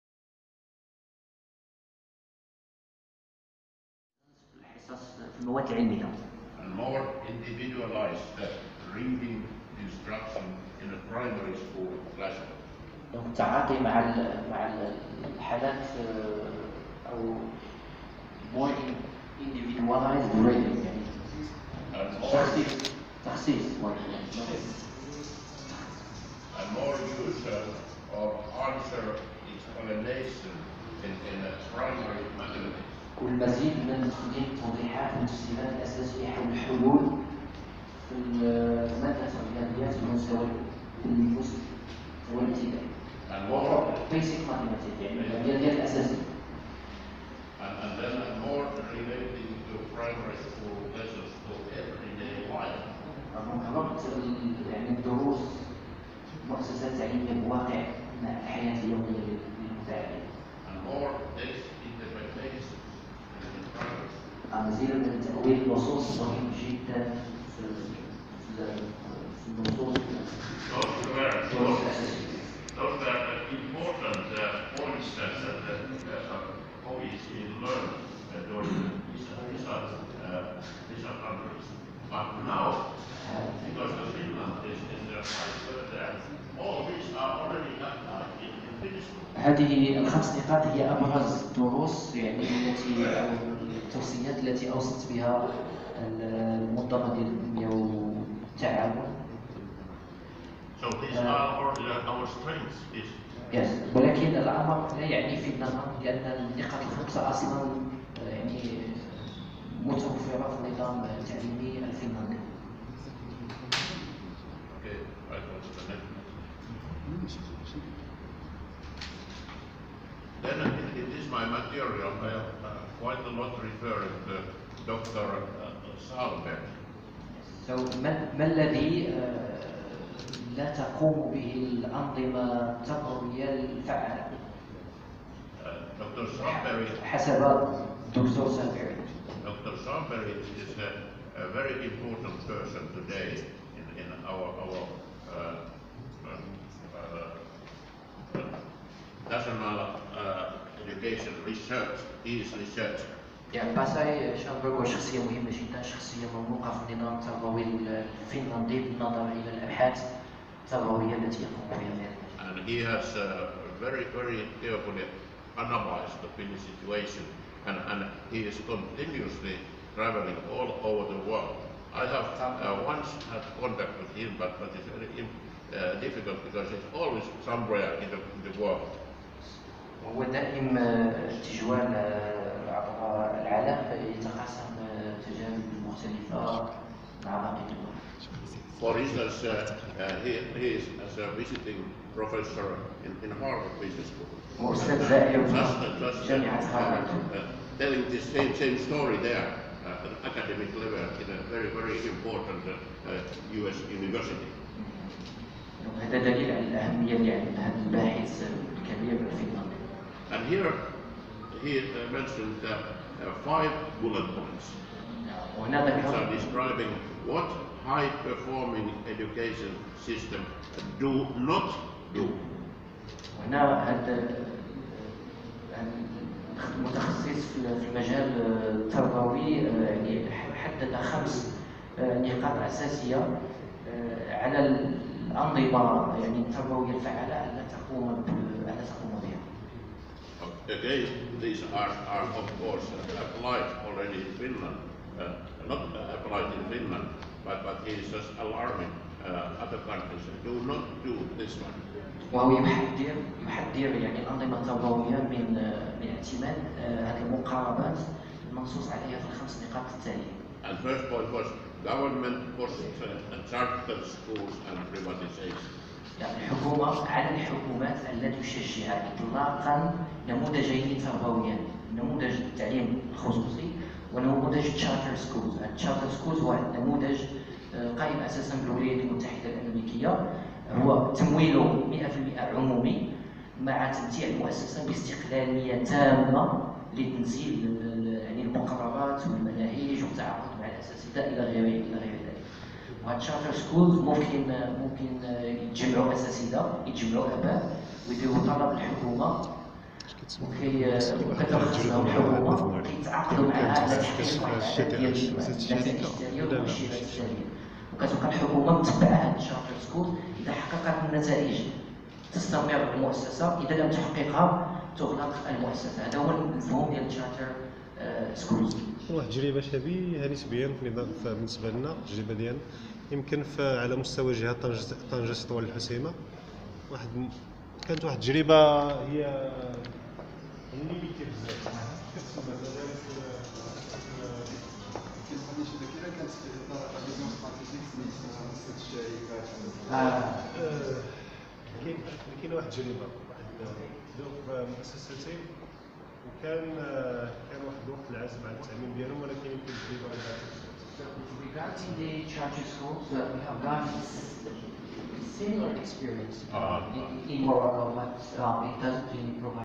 خبير تربوي فنلدي في لقاء موسع للتقاسم حول النظام التربوي الفنلندي بالاكاديمية الجهوية للتربية والت
خبير تربوي فنلدي  في لقاء  موسع للتقاسم حول النظام التربوي الفنلندي بالاكاديمية الجهوية للتربية والتكوين فاس مكناس